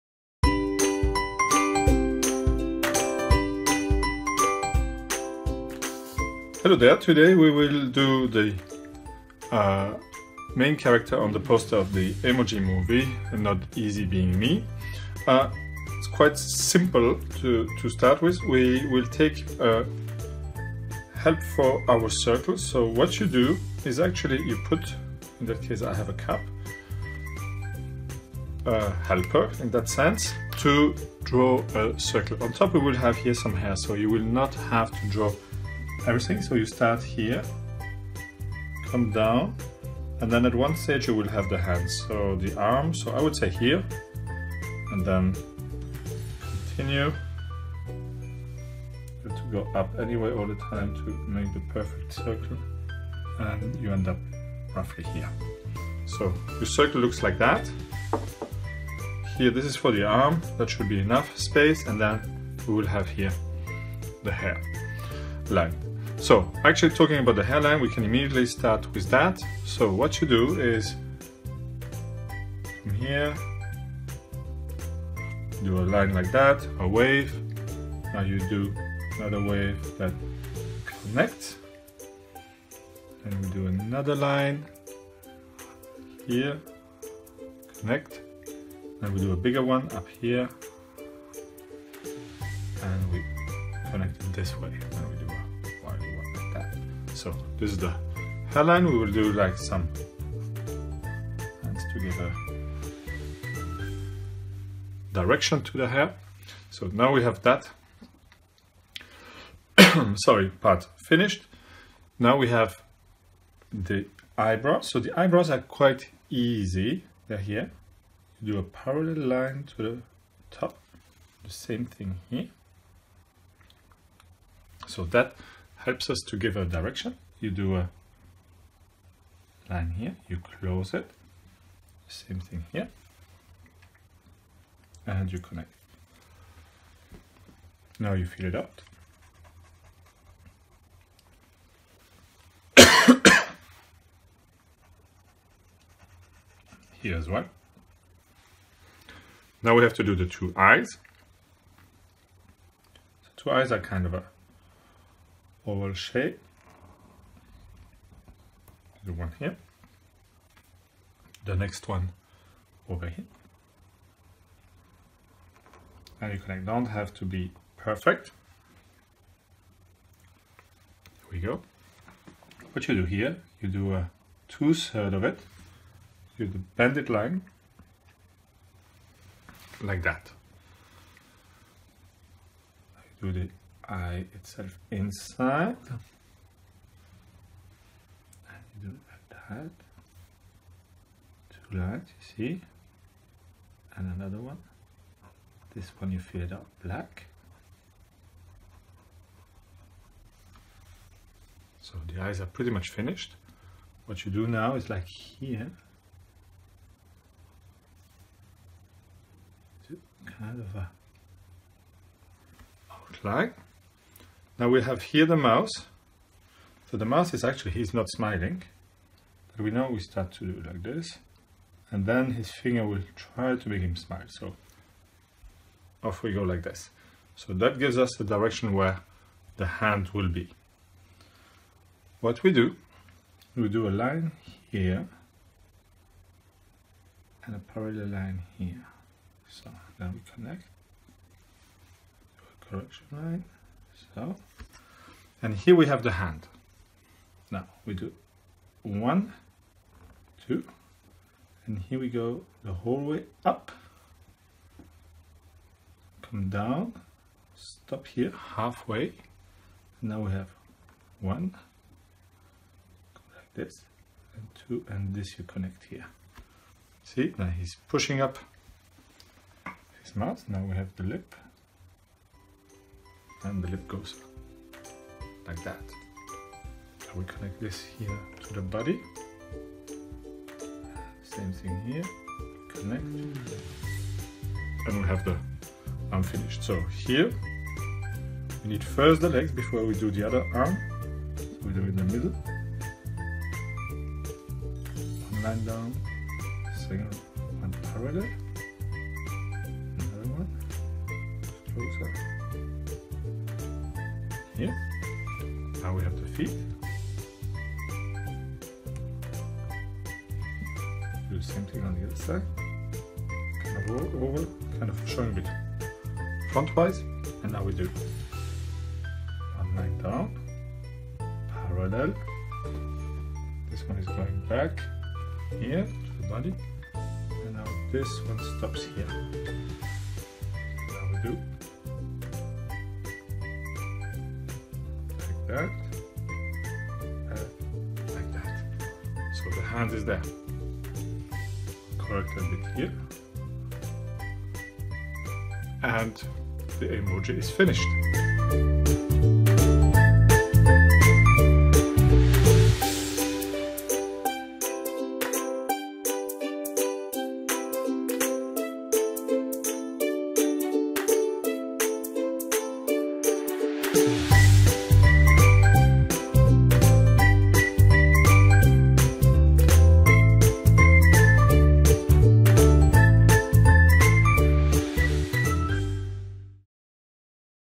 How to Draw Gene from The Emoji Movie - Easy (NARRATED)
Hi everyone, you can follow this easy step by step drawing lesson of how to draw Gene from the Emoji movie and draw this cute character in minutes.